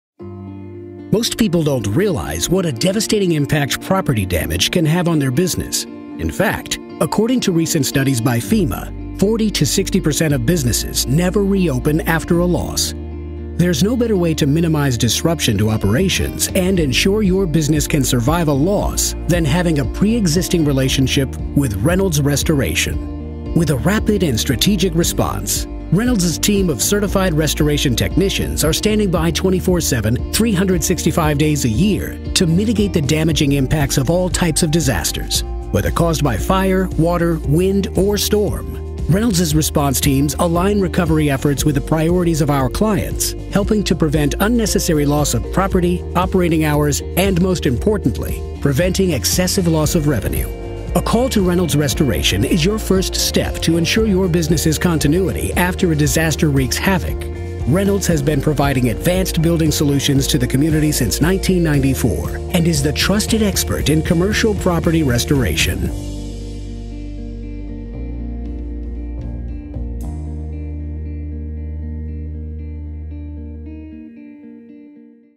Reynolds Restoration Services Commercial Services
Catastrophic events like a flood or fire can have a devastating impact on your business. There is no better way to minimize disruption to operations and ensure your business can survive a loss than having a pre-existing relationship with Reynolds Restoration Services.